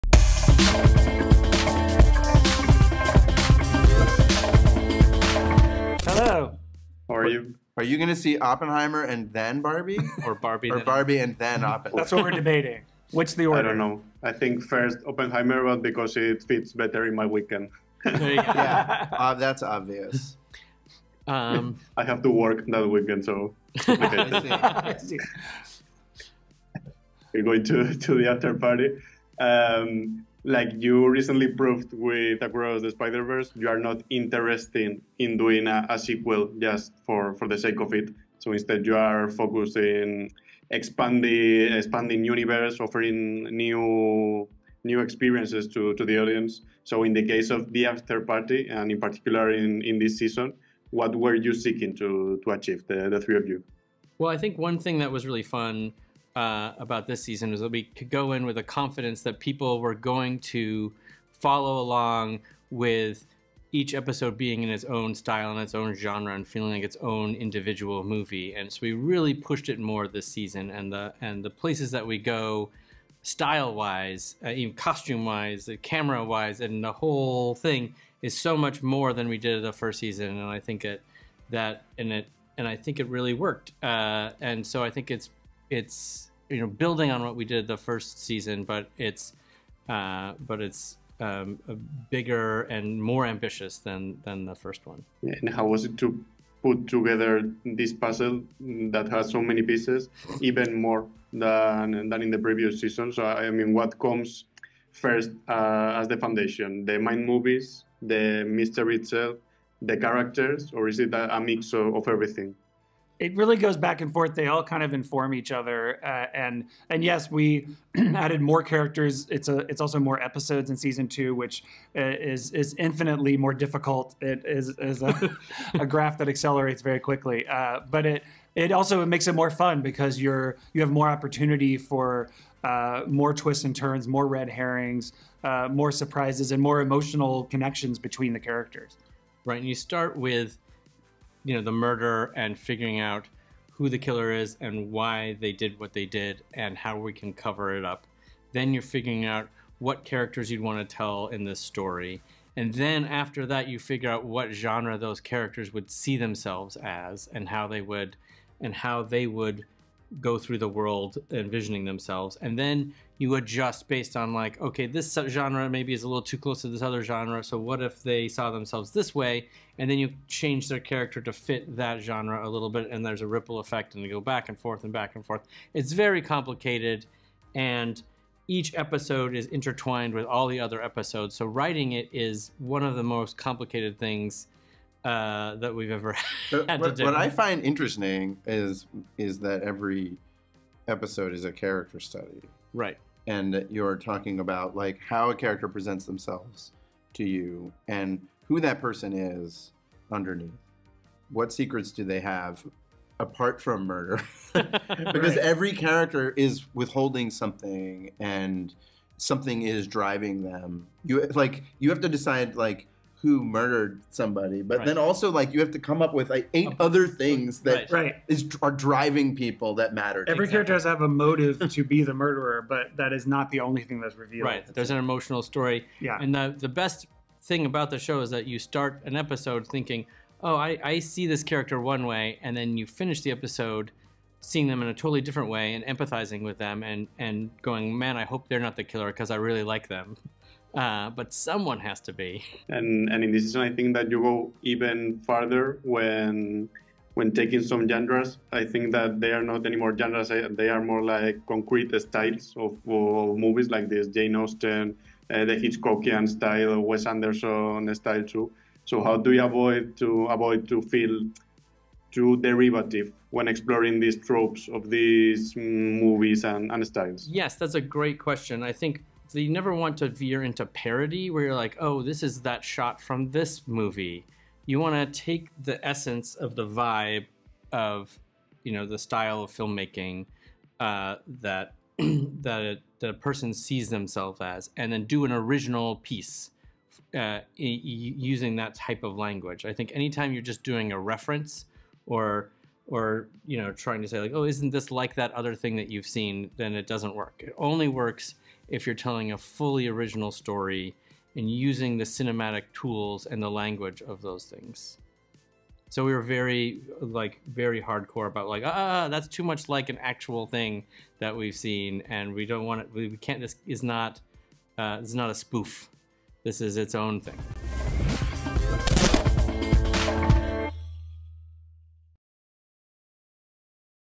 Entrevista a los responsables de THE AFTERPARTY: "La segunda temporada es más grande y ambiciosa"
Los showrunners Chris Miller y Anthony King y el guionista Phil Lord detallan cómo ha sido el proceso de creación de la segunda temporada de 'The Afterparty', la ecléctica comedia de Apple TV+.